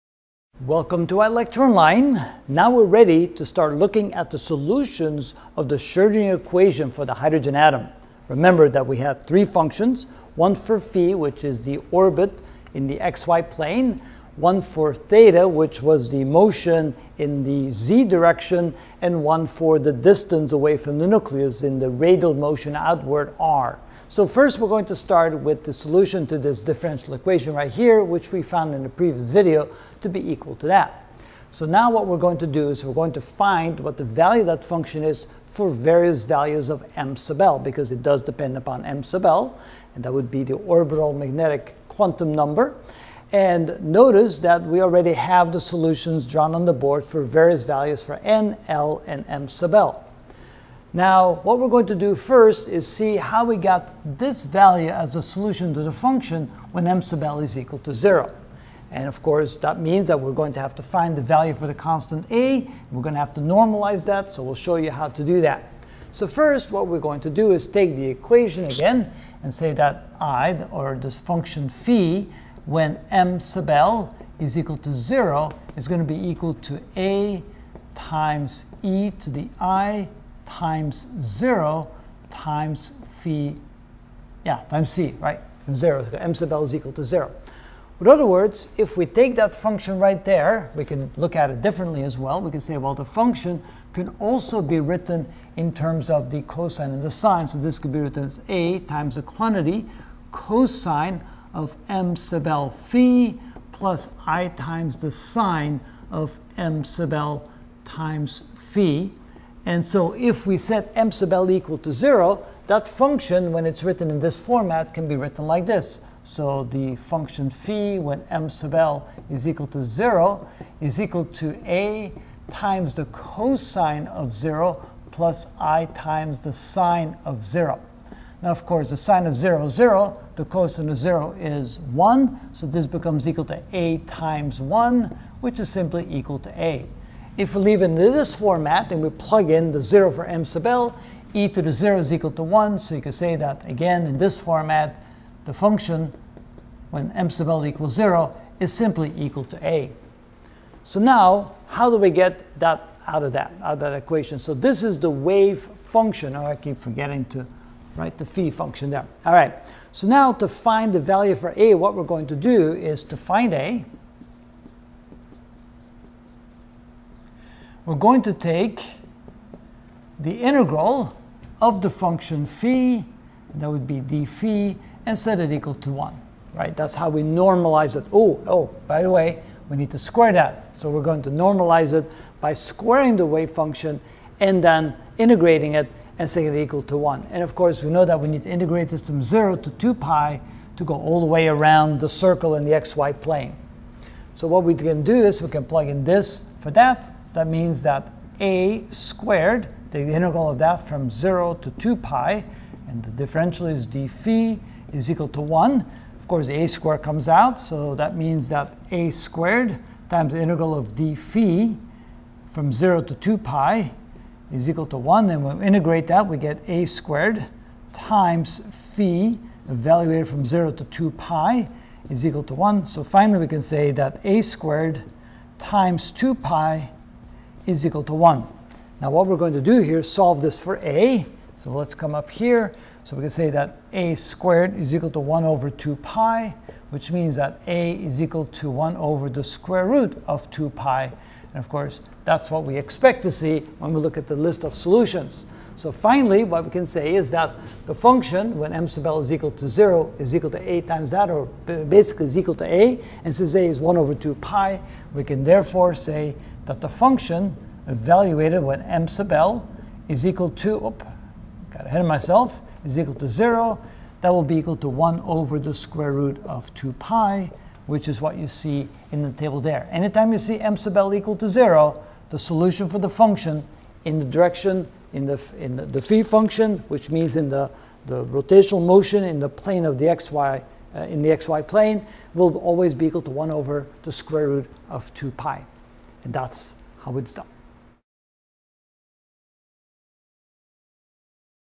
Physics - Ch 66.5 Quantum Mechanics: The Hydrogen Atom (49 of 78) Solution of the Phi Function
In this video I will find the solutions to the Schrodinger equation to the hydrogen atom. Remember there were 3 functions, the phi function which representing the orbit in the x-y plain, theta which represents the motion in the z-direction, and one that represents the distance to the nucleus.